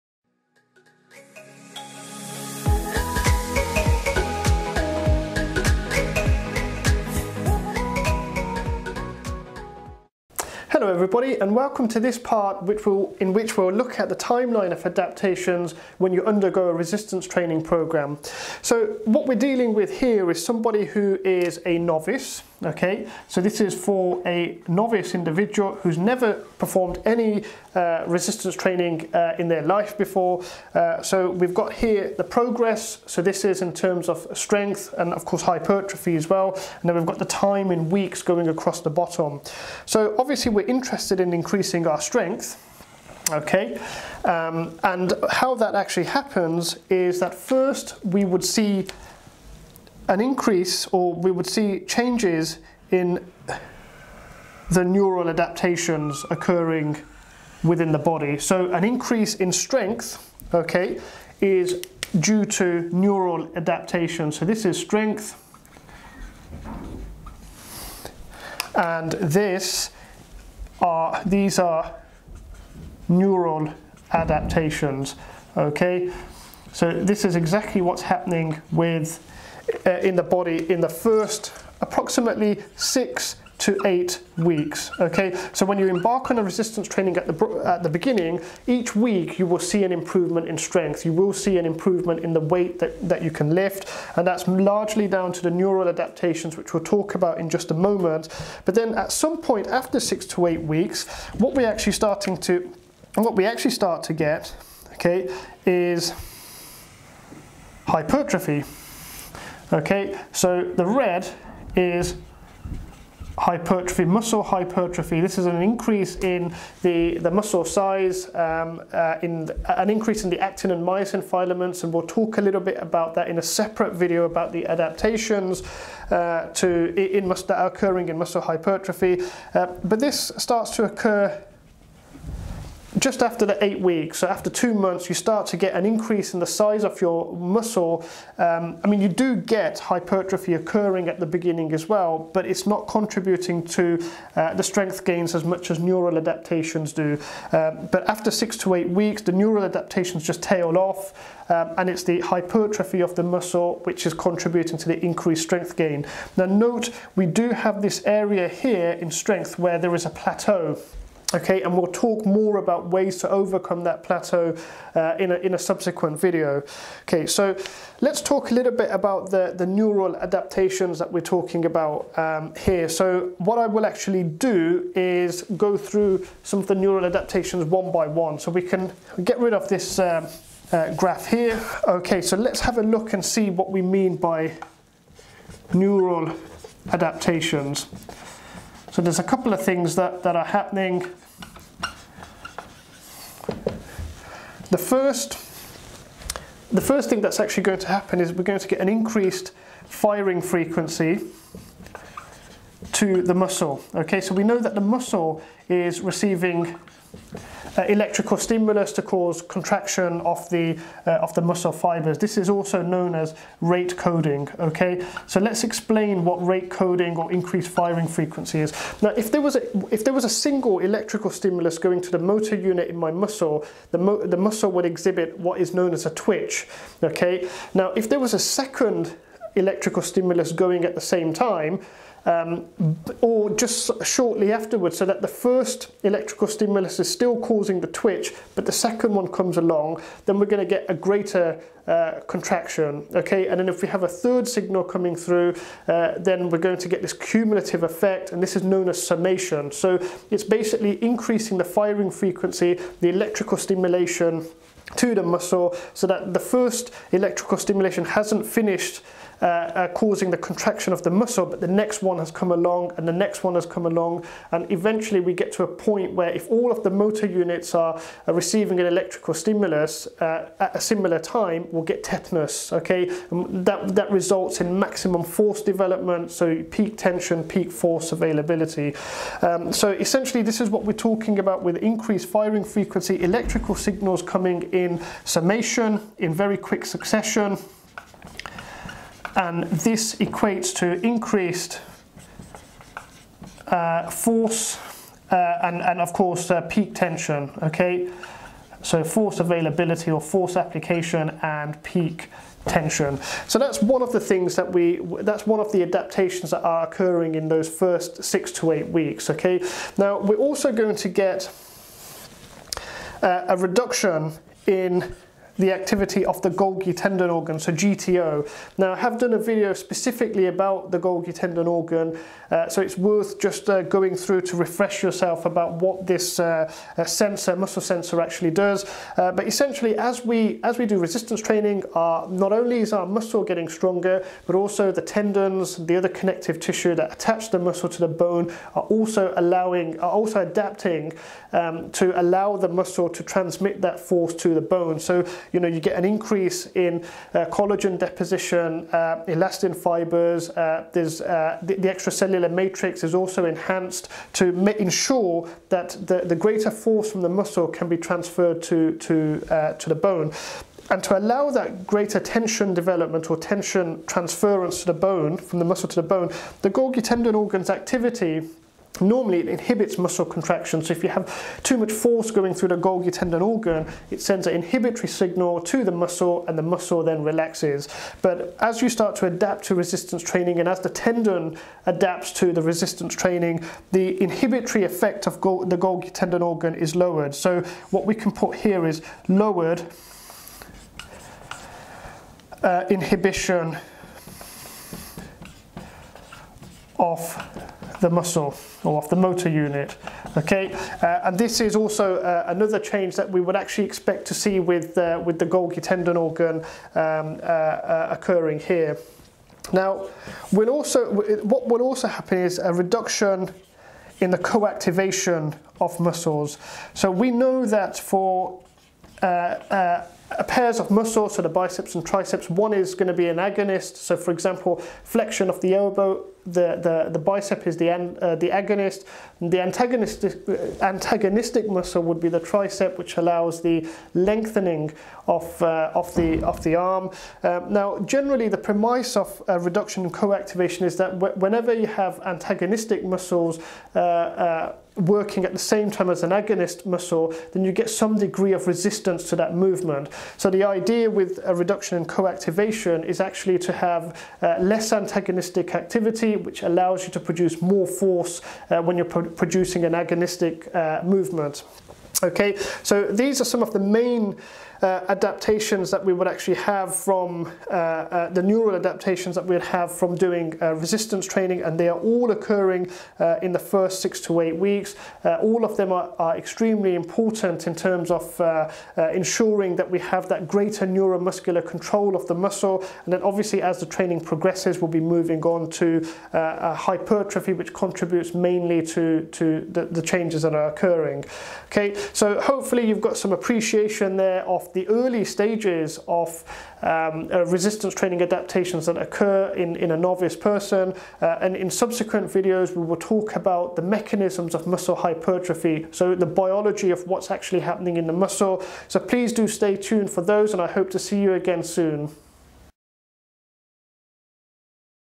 The Neural Adaptations to Resistance Training
This video tutorial explains the adaptations which occur in the neural circuitry of a novice individual newly starting a resistance training program.  The video is part of a special series about the physiological effects of resistance training.